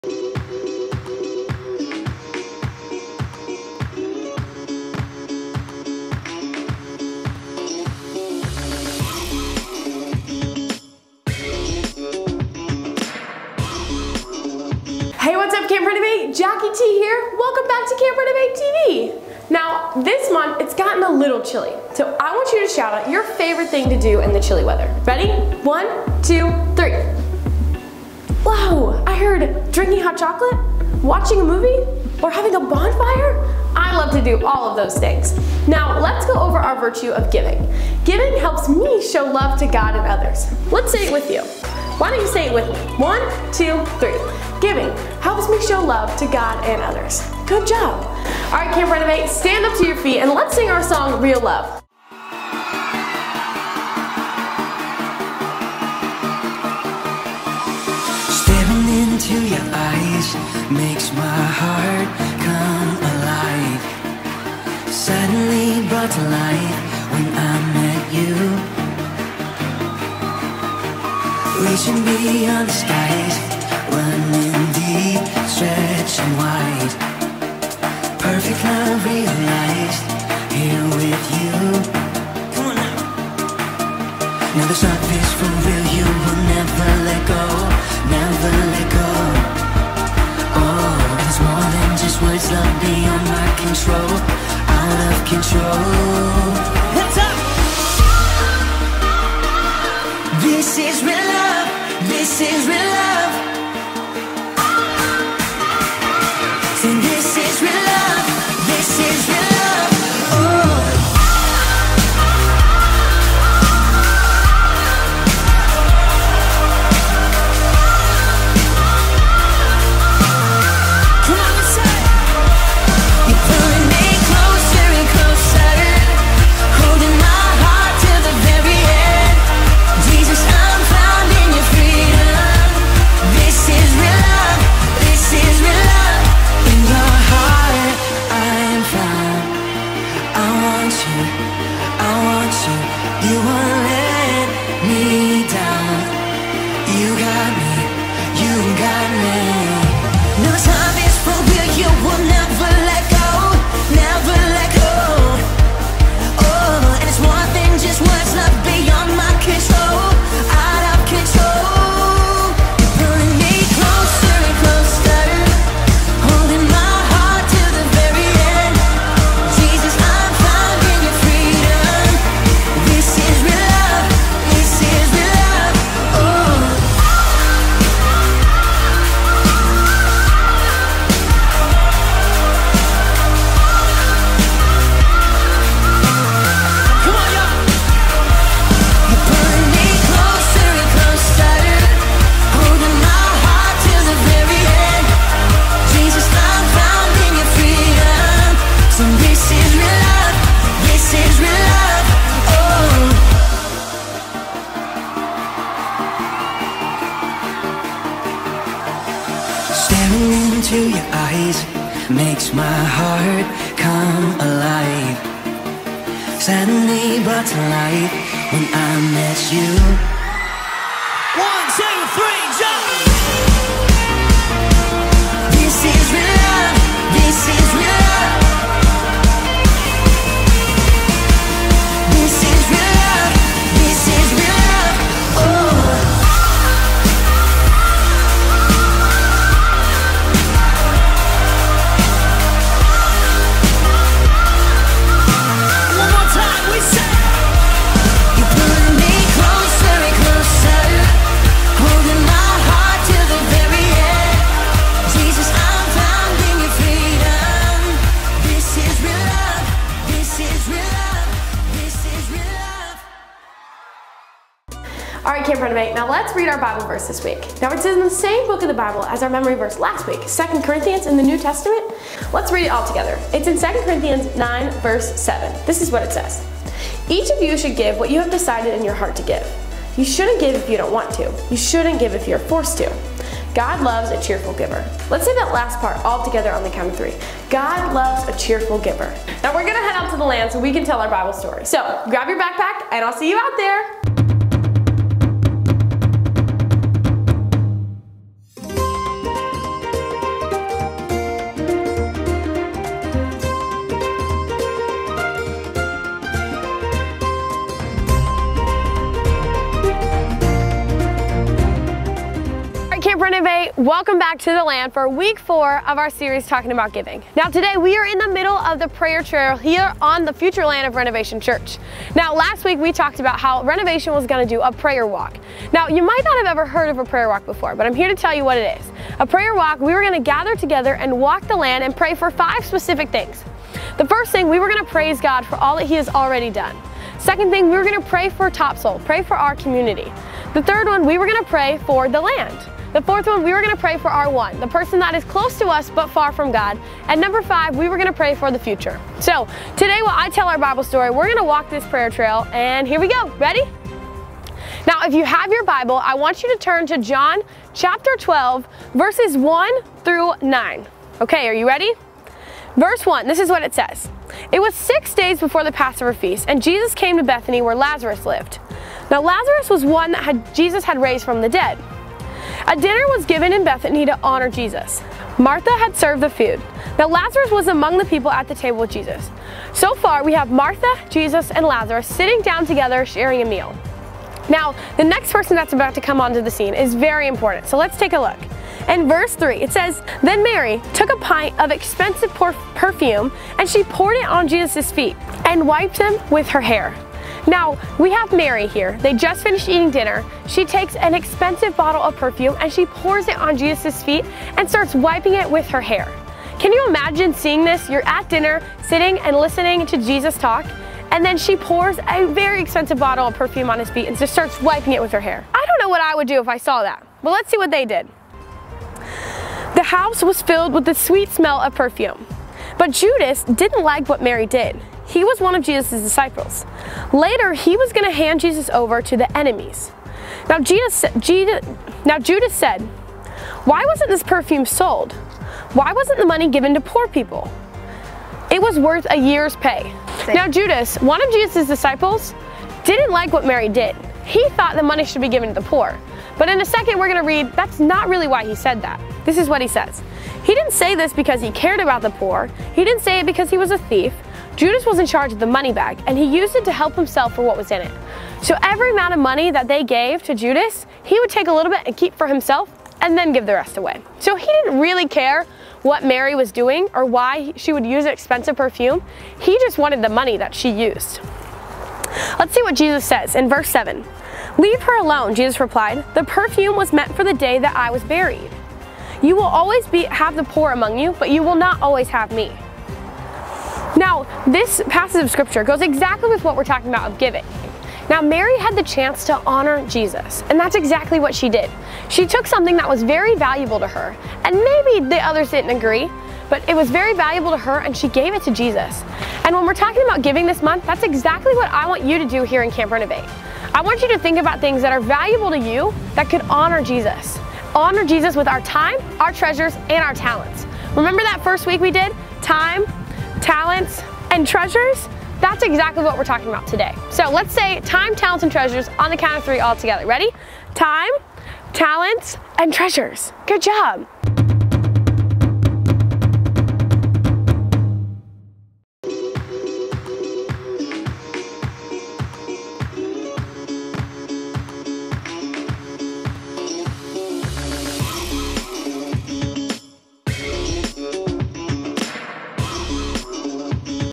Episode 038: Mary and Martha
Check out week 4 talking about GIVING. We can't to see how our kids learn to use their time, talents and treasures wisely.

[Camp Renovate is a Ministry of Renovation Church]